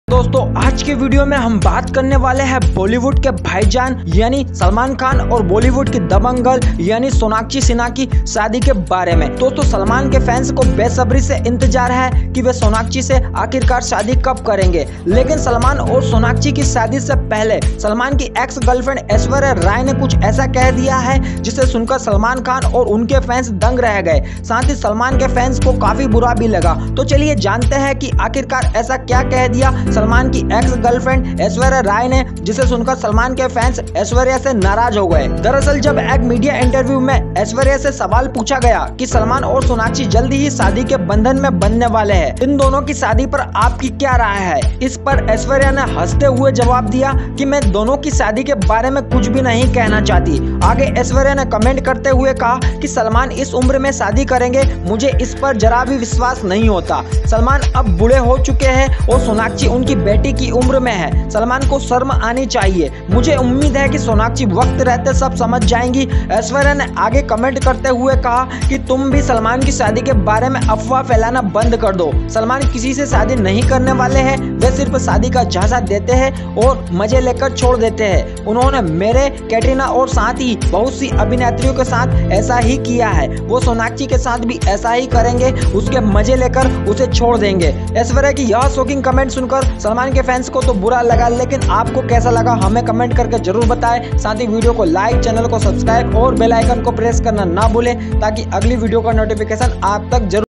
सोनाक्षी salman कि शादी को लेकर aishwarya ने ये क्या बोल दिया। salman getting marriage to sonakshi
salman सोनाक्षी की शादी को लेकर ये क्या बोल गई aishwarya rai । sonakshi getting marriage with salman
aishwarya rai ne salman khan or sonakshi sinha ki shaadi ko leke kiya esa khulaasha jankr  fans dang reh gaye